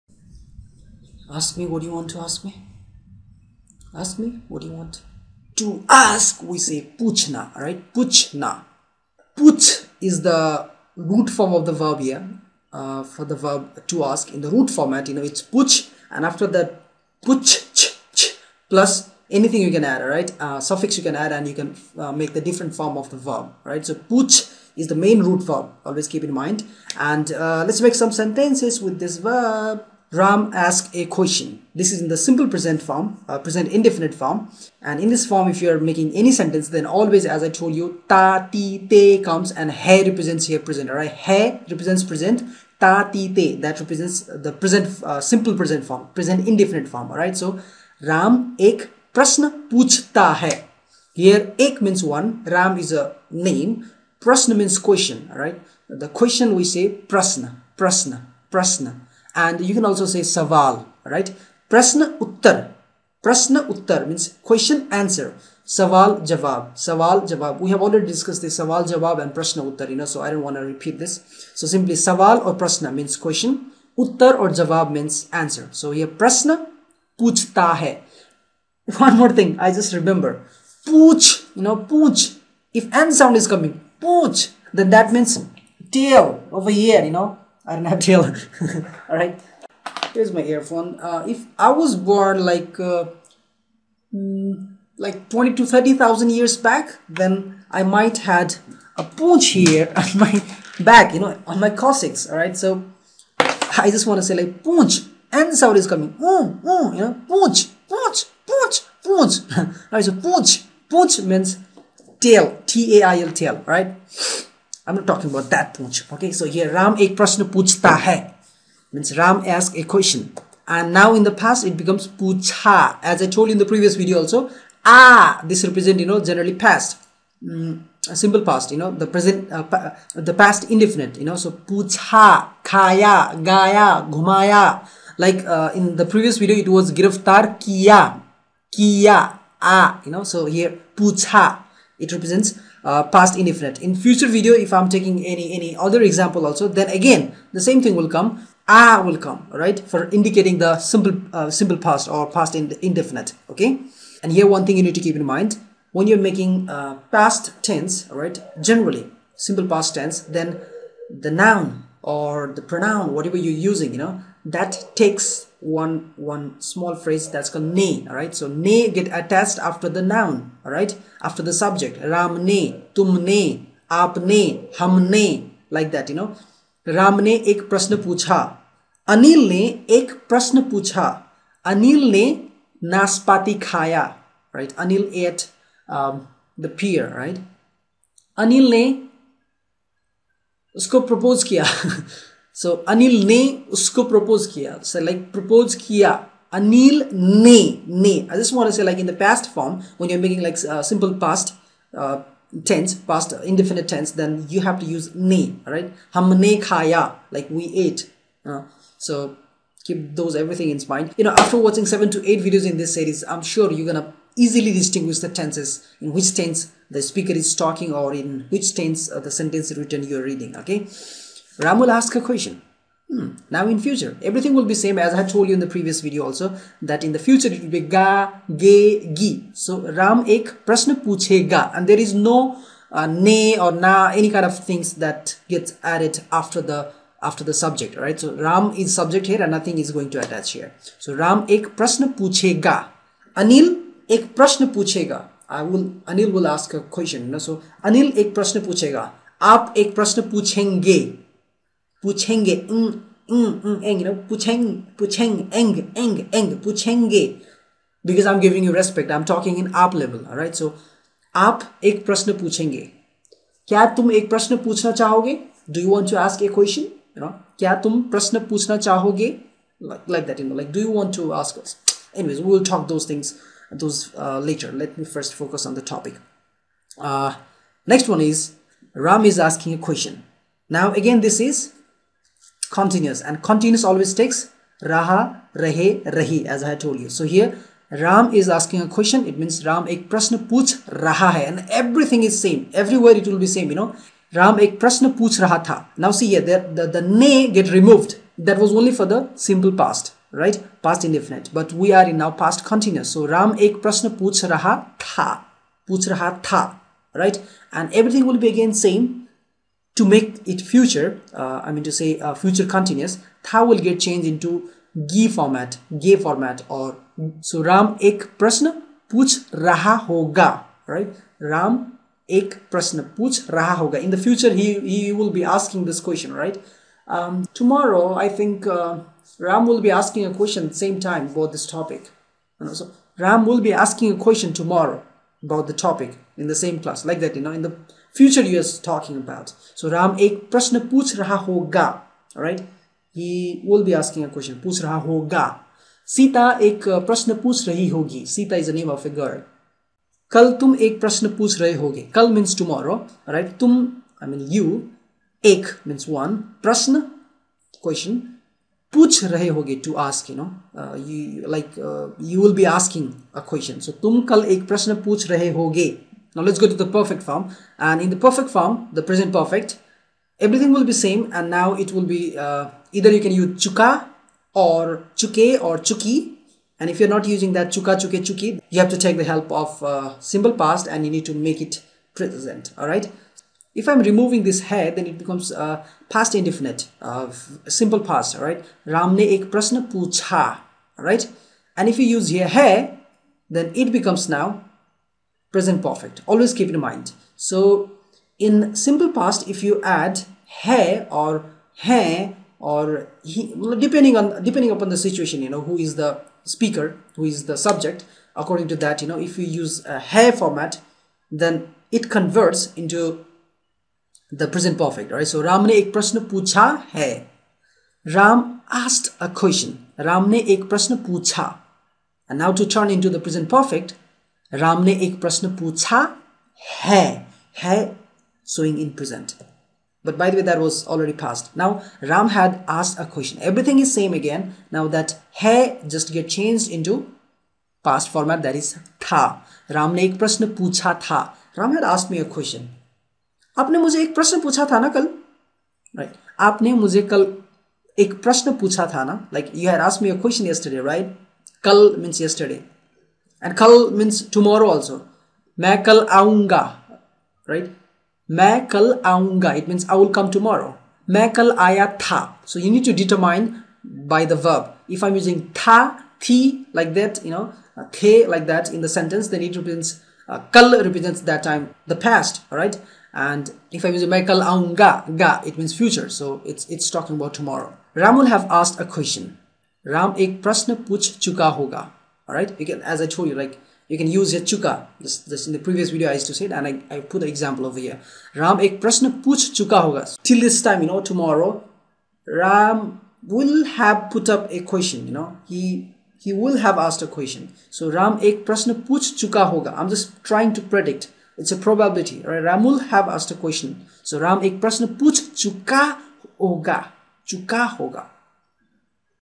Learn Hindi Grammar Videos Lessons - Learn Hindi Tenses with Verb Ask
Learn Hindi Grammar Videos Lessons - Learn Hindi Tenses with Verb "TO ASK". This is continuing video of series of learning Hindi tense with example of sentences.

 Some time learning in this way, may not be able to express exact fragrance of sentence, but this is only the easier way of learning Hindi tense for foreigners.

This Hindi Grammar Videos lessons contains 9 tense structure.
1. Simple Present ( Present Indefinite)
2. Simple Past ( Past Indefinite)
3. Simple Future ( Future Indefinite)

4. Present continuous
5. Past continuous
7. Future Continuous.

8. Present perfect
9. Past Perfect
9. Future Perfect.

I have covered this tense sentence structures, as these are the most tenses to be learnt . Other we will cover in later videos.

Present Indefinite sentence always ends with - ta/ti/te + Hai/ Hain/Hoon
Past  Indefinite sentence always ends with - aa sound ( mostly)
Future Indefinite sentence always ends with - ga,gee, ge

Continuous Tense always include before the verb - rha/ rhe/rhi

And

Present Continuous Tense structure is like =  rha/ rhe/rhi + hai/hain/hoon

Past Continuous Tense structure is like =  rha/ rhe/rhi + tha/thi/then

Future Continuous Tense structure is like =  rha/ rhe/rhi + hoga/hogee/honge

Based on these Hindi Tense sentence structure we are learning to make Hindi sentences.